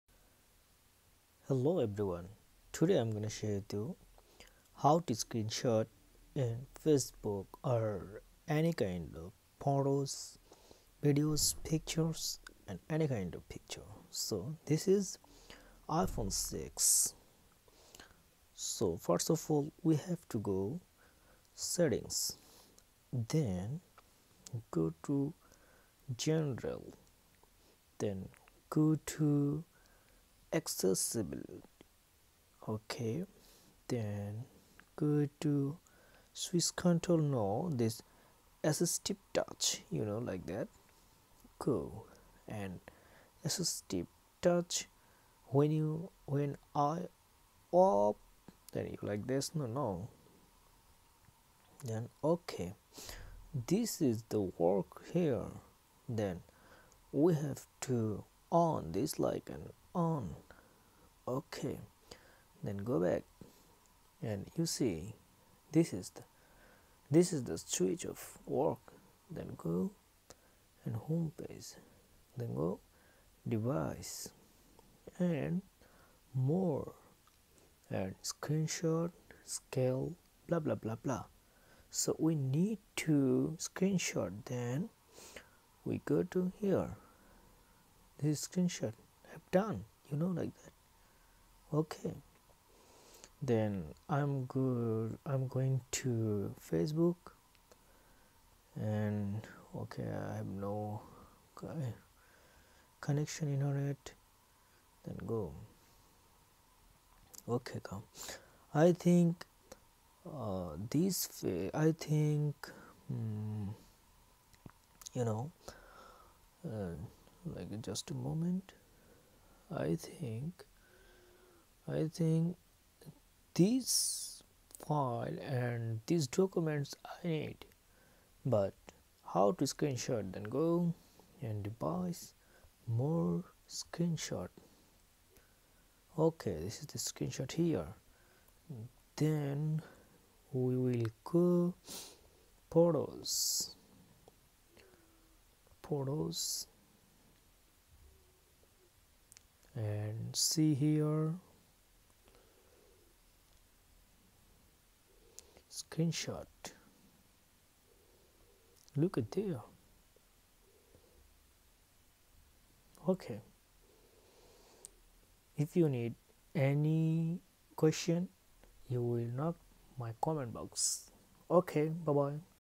how to screen short iphon6/all iphone
SUP WORLDS!
This video will show you how to record your iphone screen? in iOS 11.
Let me know if there is another tutorial besides iOS 11 screen recording that you Worlds would want me to do.
This video will show you how to record your iPhone 8 screen.
This video will also show you how to record your iPhone X screen.